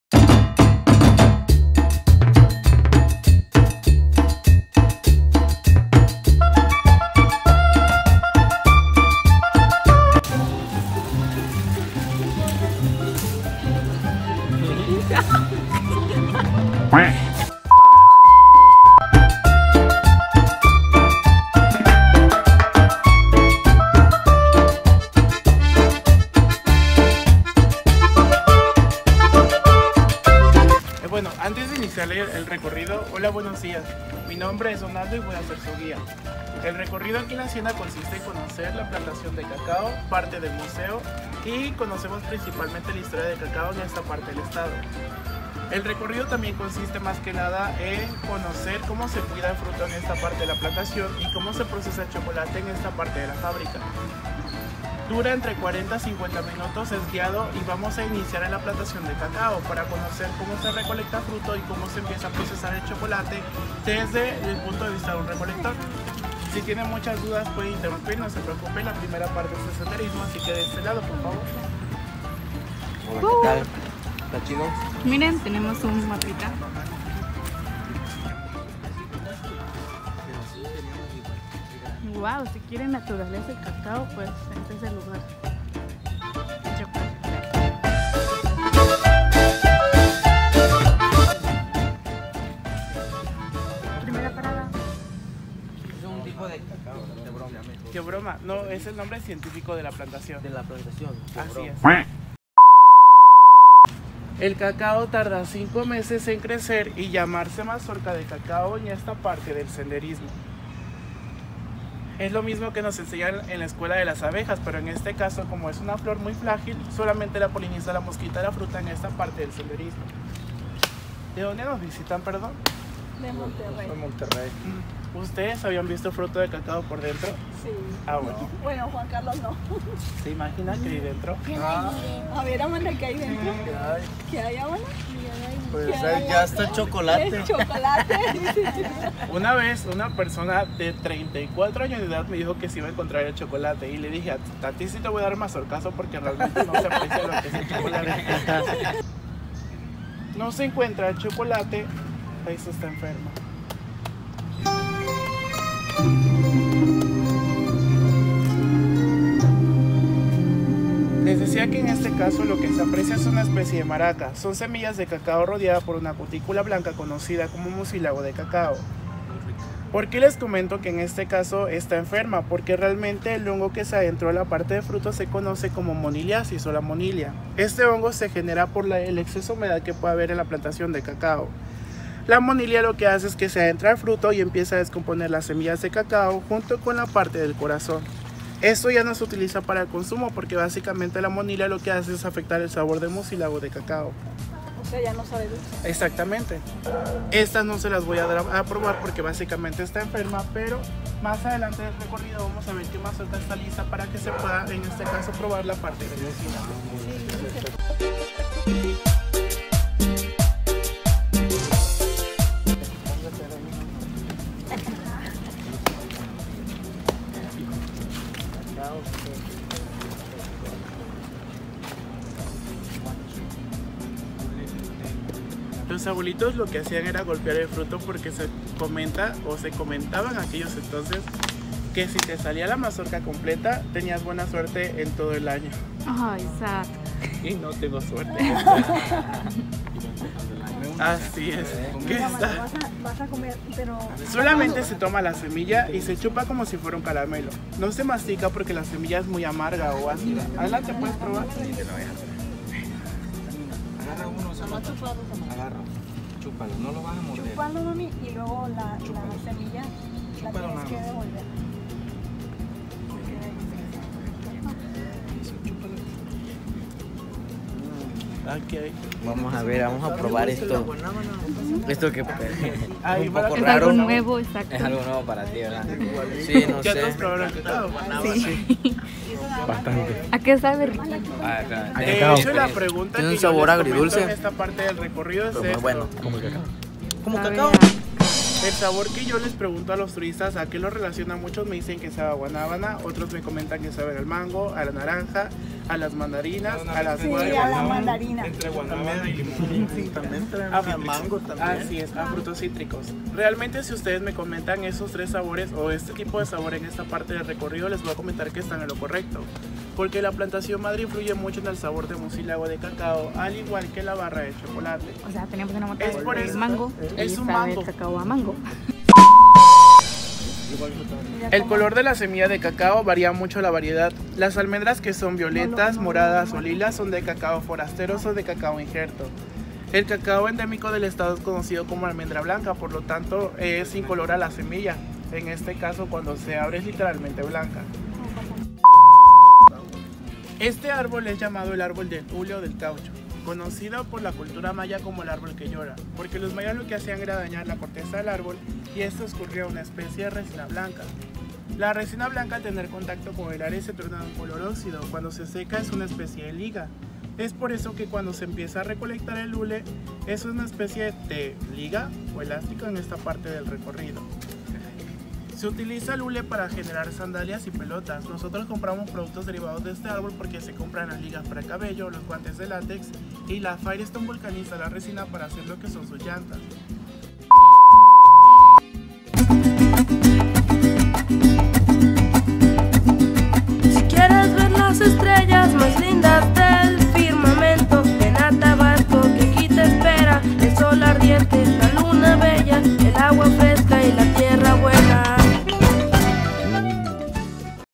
HACIENDA LA LUZ| Comalcalco, Tab.| Segunda parte
Hola amigos, aquí les dejamos la segunda parte de nuestro recorrido que consiste e el senderismo por toda la plantación de cacao. Esparamos les guste... Y no olviden seguirnos en nuestro instagram: